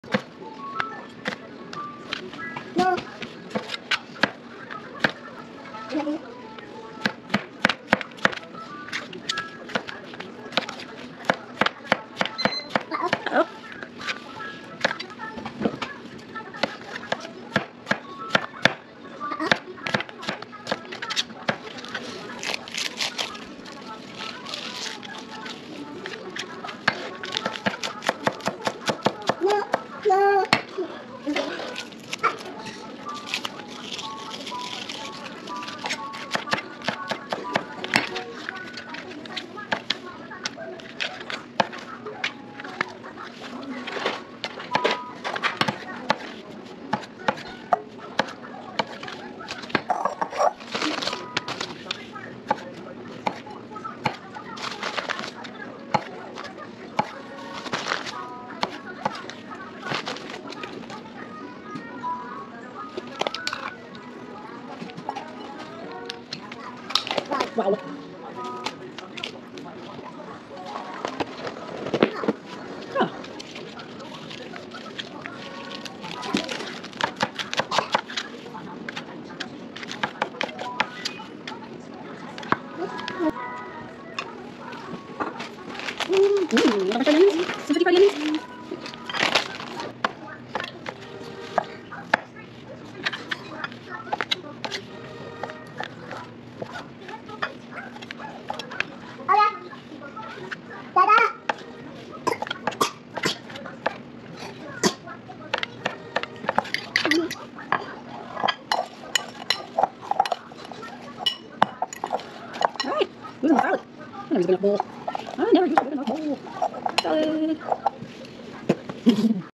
Let's Chop a Salad
Basically all of the produce came from our Farm Box from @farmfreshcarolinas3622 check them out!

We'll have another video of unboxing and some quick farm box recipes coming soon!

Noosheh jaan ~ Enjoy!